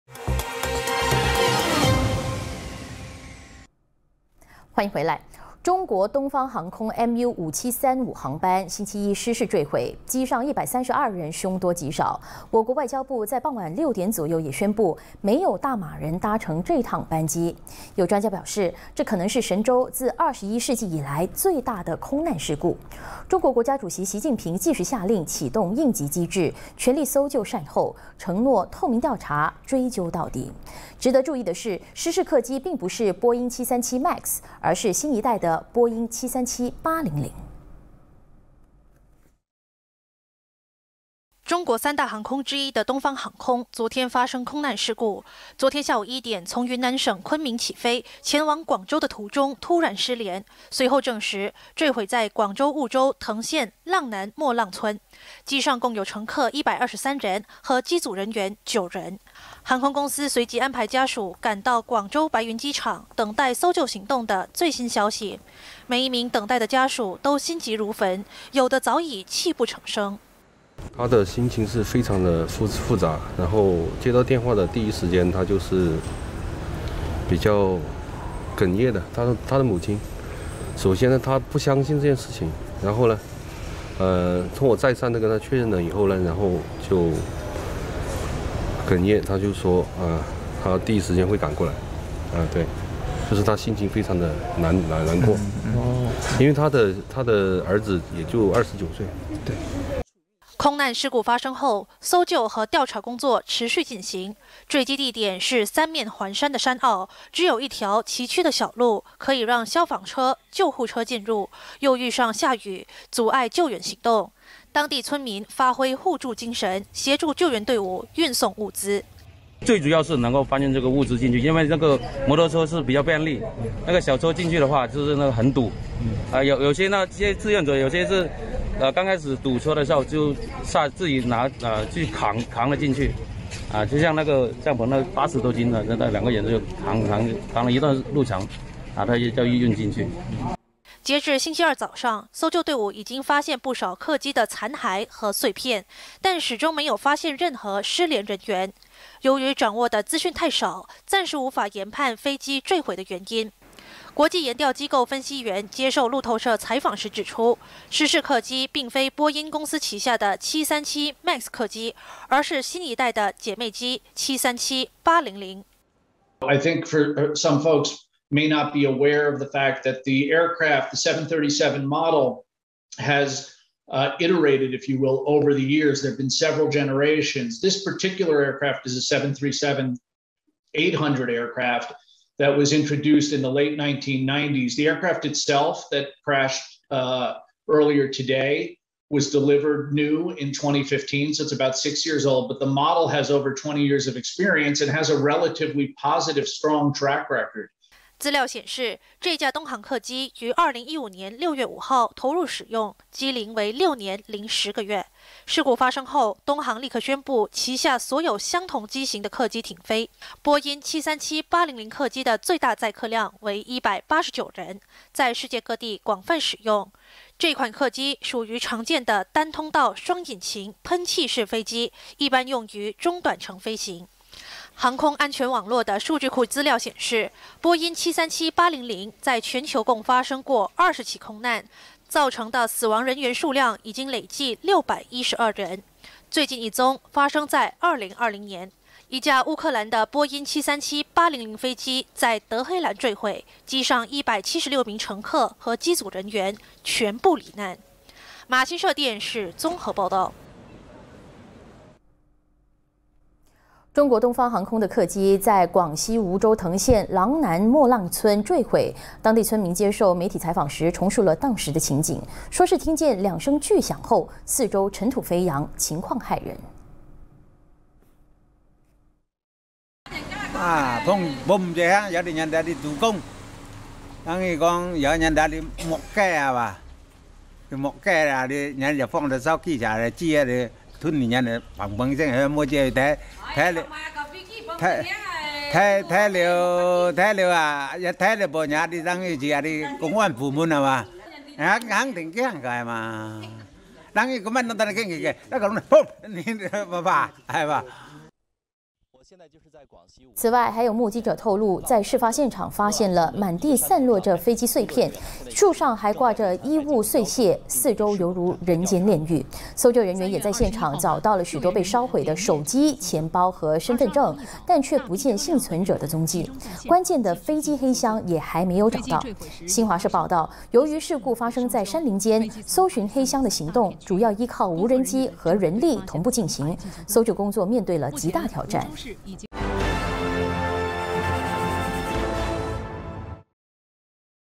20220322 神州21世纪最大空难 东航132人恐无一幸免于难
中国东方航空MU5735航班星期一失事坠毁，机上132人凶多吉少。有专家表示这可能是神州自21世纪以来最大的空难事故，中国国家主席习近平即时下令启动应急机制，全力搜救善后，承诺透明调查追究到底。

值得注意的是，失事客机并不是波音737MAX而是新一代的波音737-800。